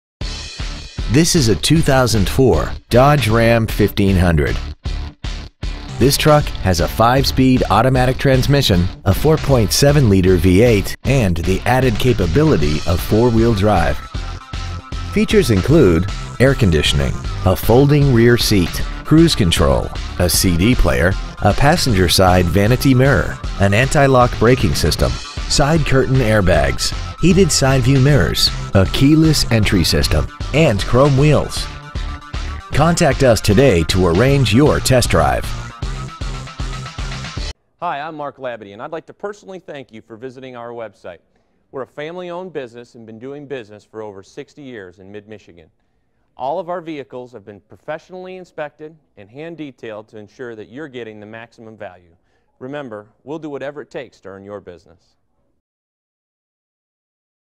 Used 2004 Dodge Ram 1500 Bay City MI - SOLD
Year : 2004
 Make : Dodge
 Model : Ram 1500
 Engine : Gas V8 4.7L/287
 Trans . : Automatic
 Exterior : Bright Silver Metallic
 Miles : 84,151
 Interior : Dark Slate Gray w/Cloth 40/20/40 Split Bench Seat
 Stock : T09313B

 Used 2004 Dodge Ram 1500 Bay City MI